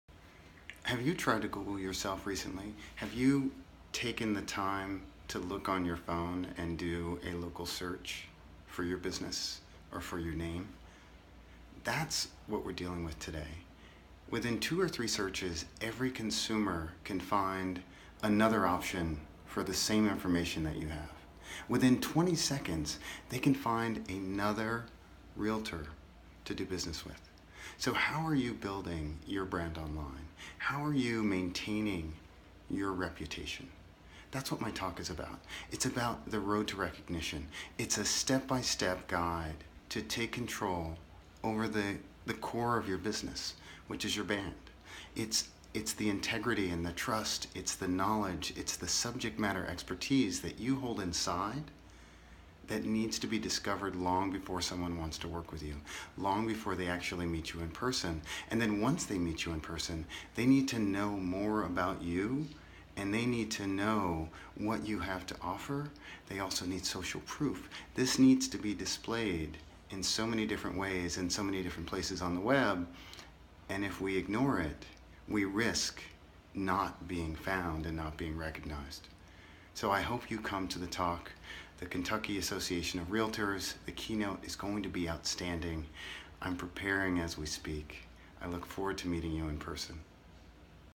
Kentucky Association of REALTORS Convention - Seth Price
Seth Price, keynote speaker at the Kentucky Association of REALTORS (KAR) Convention & Expo, gives attendees some insight into what his talk will cover.